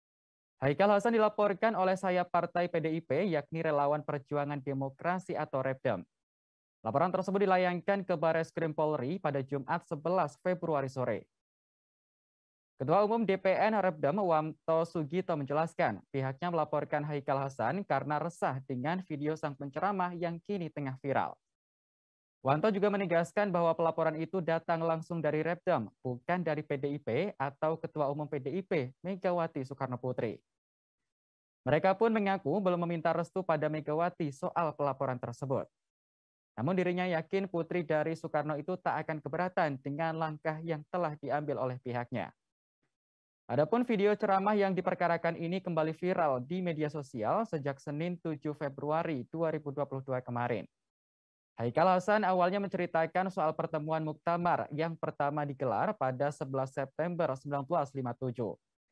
Sebut Bung Karno Tukang Penjarakan Ulama, Penceramah Haikal Hassan Dilaporkan ke Bareskrim Polri
TRIBUN-VIDEO.COM - Penceramah Haikal Hassan dilaporkan ke polisi atas dugaan penghinaan terhadap Presiden pertama RI, Ir Soekarno.

Dalam video ceramahnya, Haikal Hassan menyebut bahwa Bung Karno adalah tukang memenjarakan ulama.

Haikal Hassan dilaporkan oleh sayap Partai PDIP, yakni Relawan Perjuangan Demokrasi (Repdem).

Laporan tersebut dilayangkan ke Bareskrim Polri pada Jumat (11/2) sore.

Ketum DPN Repdem Wanto Sugito menjelaskan, pihaknya melaporkan Haikal Hassan karena resah dengan video sang penceramah yang kini tengah viral.

Wanto juga menegaskan bahwa pelaporan itu datang langsung dari Repdem, bukan dari PDIP atau Ketua Umum PDIP Megawati Soekarnoputri.

Mereka pun mengaku belum meminta restu pada Megawati soal pelaporan tersebut.

Namun ia yakin, putri dari Soekarno itu tak akan keberatan dengan langkah yang diambil oleh pihaknya.

Haikal Hassan awalnya menceritakan soal pertemuan muktamar (ijtima') yang pertama digelar pada 11 September 1957.

Menurutnya, muktamar ulama itu digelar di Palembang dan dihadiri banyak tokoh agama.

Di antaranya adalah Buya Hamka, Muhammad Natsir, Syahrir hingga Kahar Mudzakkar.

Saat itulah, Haikal Hassan menyebut bahwa Bung Karno bersama tokoh PNI, PKI dan Nasakom kemudian menghina para ulama yang sedang melangsungkan muktamar.

Menurutnya, Bung Karno dan sejumlah tokoh partai itu telah menuduh ulama serta tokoh nasional yang hadir sebagai orang yang tidak bermoral (amoral).

Tak hanya itu, Haikal Hassan juga menuding Bung Karno tukang memenjarakan para ulama.